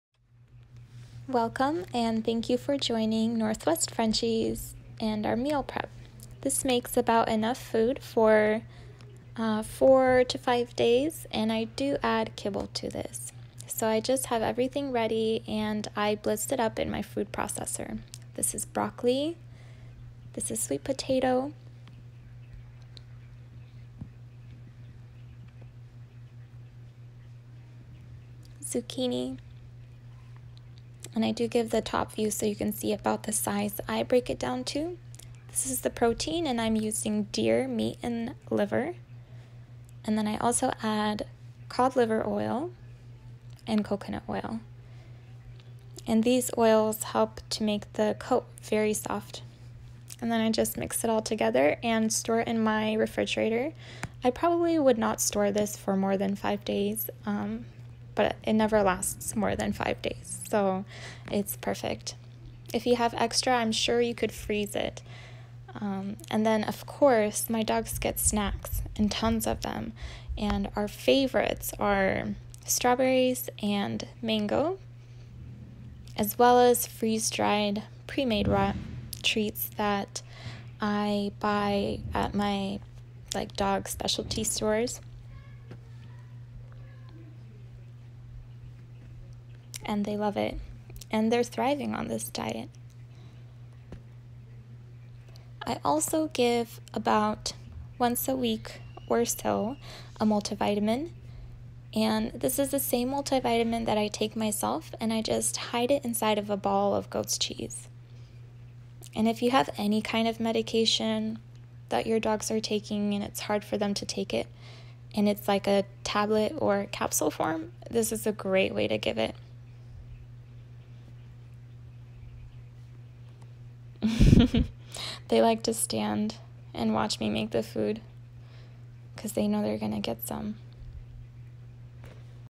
Meal Prep | What my Dogs Eat in a Day | NW Frenchies Holistic Home Made Diet for Optimal Health
Hope you find some inspiration to do the best that you can do for your furry family member at home. My French Bulldogs have soft & beautiful coats, clean white teeth, no bad breath, no mushy poops, and tons of energy. My Frenchies would rather starve than eat plain dry kibble. I have been cooking for my dogs ever since I have owned them. I still do add a small amount of kibble to this food to bulk it up however, I am hoping to soon get off of kibble altogether. Fresh and lighly cooked food has more nutrients (vitamins, minerals, phytonutrients, live enzymes). Kibble is highly processed and not an evolutionarily appropriate diet for canines. I change up the ingredients so that they receive different type of nutrients and don’t get tired of eating the same thing every time I make food for my dog. Today I used boiled deer meat and liver, raw zuchinni, raw sweet potato, steamed broccoli, fish oil, and coconut oil. This diet is great for young adult, adult, and pregnant dogs. For breastfeeding mom, weaning puppies, and young puppies I feed slightly different food to account for increase calorie and nutrient needs.

The freeze dried raw meat treats my Frenchies are currently loving can be purchased

and more!